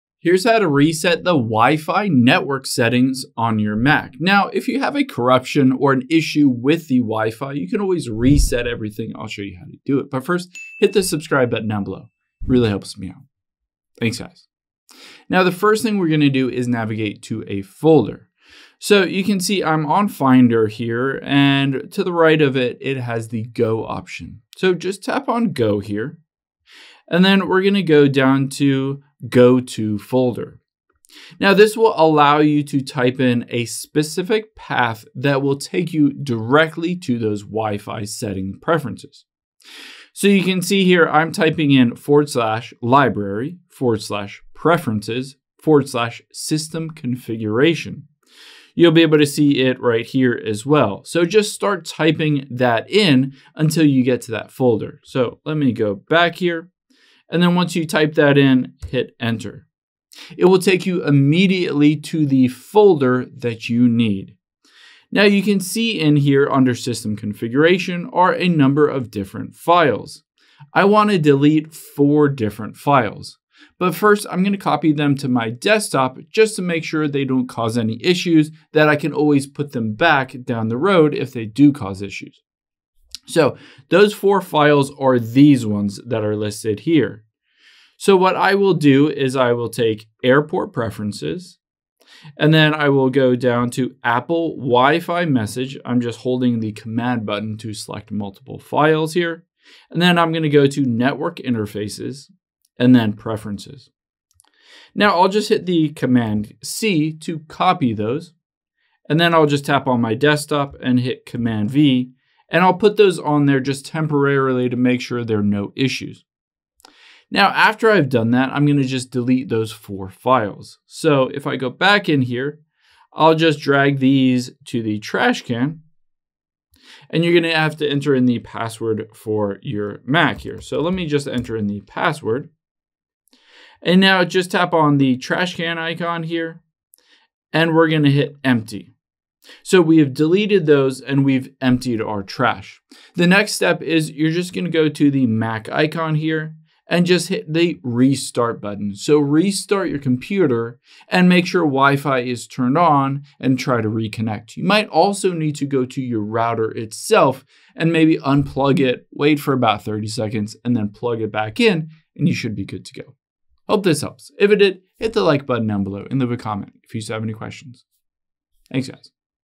How To Reset WiFi Network Settings On Mac
It's easy to reset the WiFi and network settings on your Mac if you're having connectivity issues.